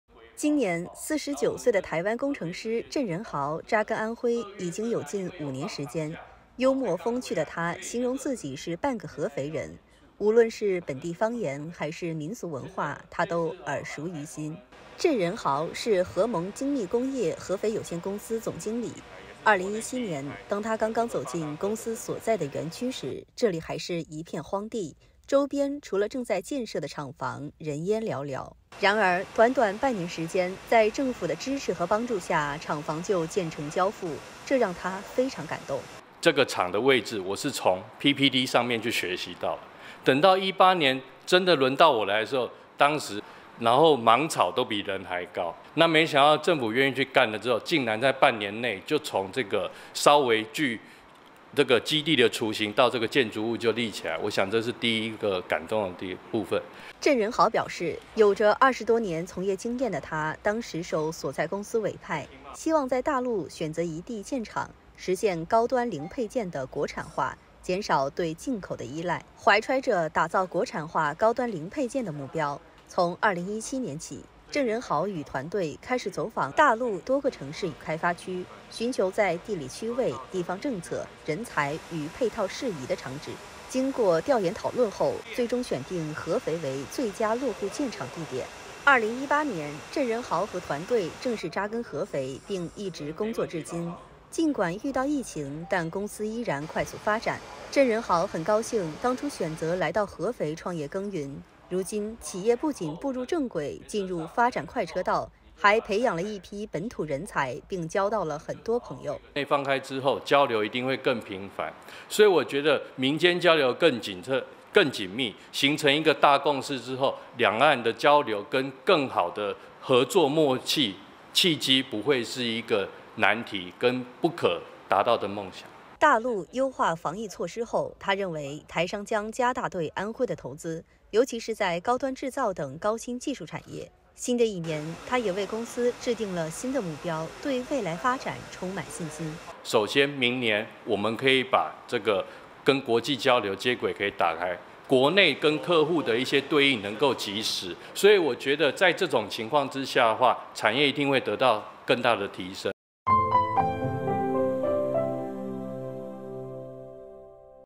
台湾工程师扎根安徽：对两岸交流和未来发展充满信心
今年49岁的台湾工程师郑人豪扎根安徽已经有近5年时间，幽默风趣的他形容自己是半个合肥人，无论是本地方言，还是民俗文化，他都耳熟于心。

郑人豪是合盟精密工业(合肥)有限公司总经理，2017年，当他刚刚走进公司所在的园区时，这里还是一片荒地，周边除了正在建设的厂房，人烟寥寥。然而短短半年时间，在政府的支持和帮助下，厂房就建成交付，这让他非常感动。（张俊）